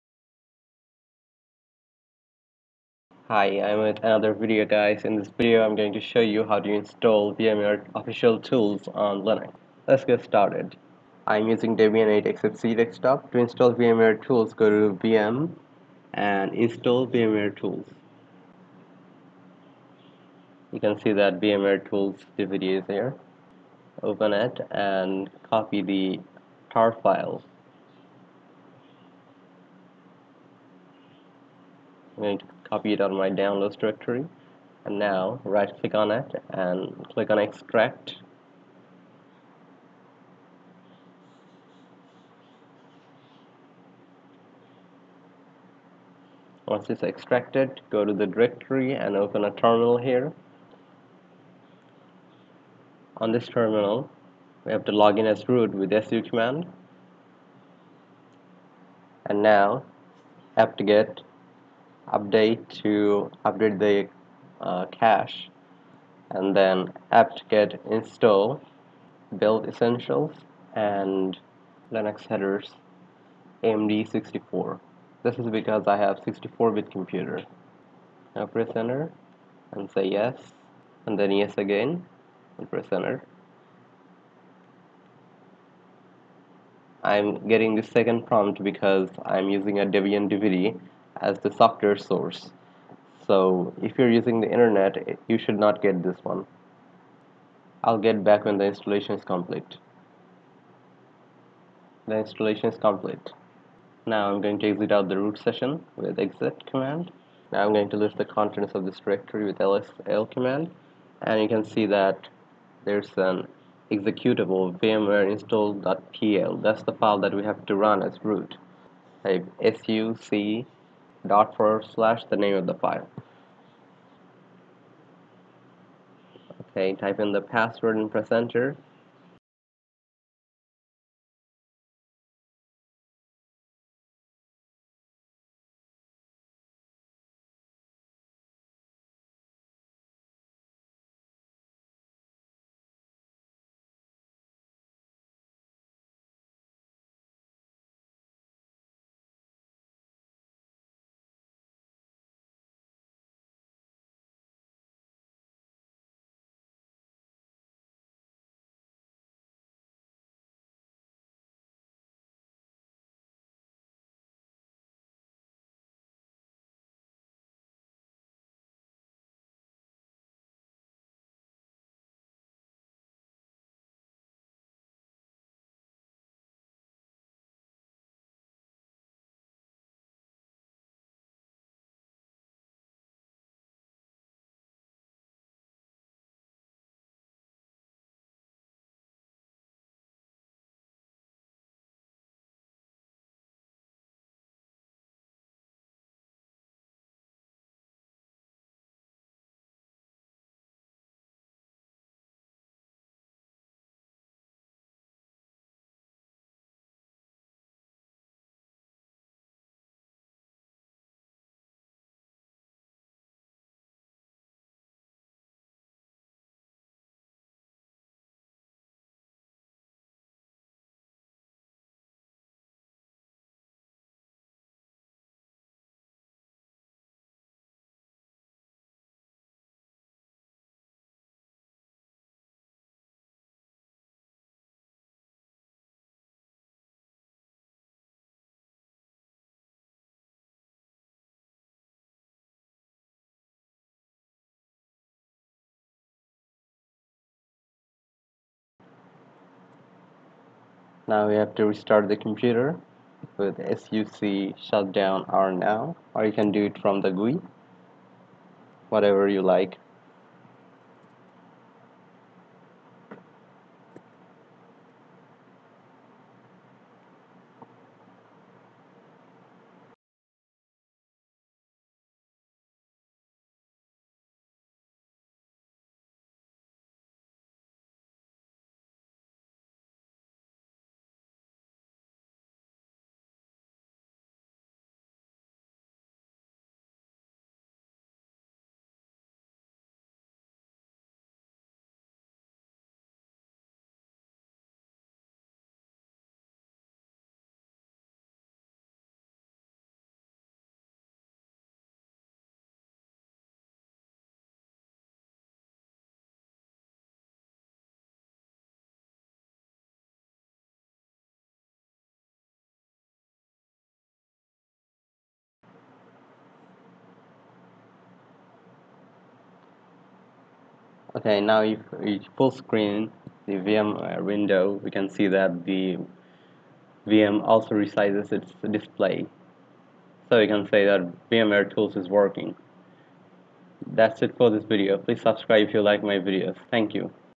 Install Official VMware Tools on Ubuntu/Debian
In this video, I demonstrated the installation procedures of VMware tools on Debian. The same procedures work on Ubuntu. I used Debian 8 Jessie for the demonstration. It should work the same on CentOS 7 and RHEL 7, the only difference is the the commands you run to install gcc and linux-headers.

Commands:

# install build tools (linux headers and gcc compiler)
sudo apt-get update
sudo apt-get install build-essential linux-headers-amd64

# Run the vmware installer as root
sudo ./vmware-install.pl

You can also use open-vm-tools. I will do a test on that in another video.
But you can see how it works for yourself.
# Install open vm tools
sudo apt-get update
sudo apt-get install open-vm-tools open-vm-tools-desktop